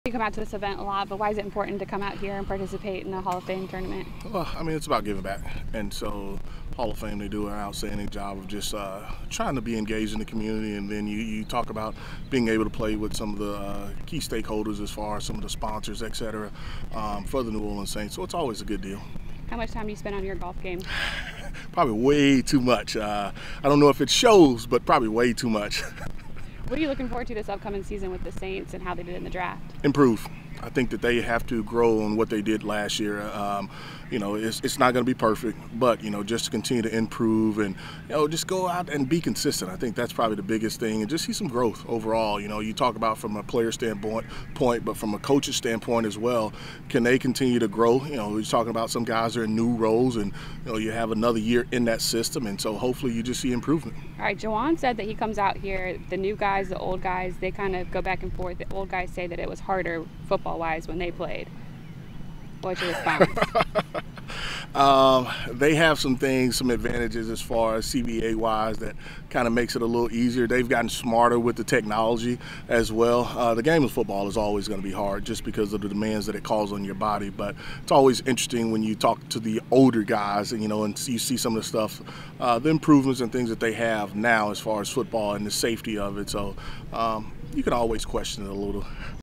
Deuce McAllister on Saints Priorities in 2023 | Saints Hall of Fame Golf Classic 2023
New Orleans Saints legend running back Deuce McAllister speaks with team reporter Erin Summers at the Saints Hall of Fame Golf Classic on Monday, May 15, 2023.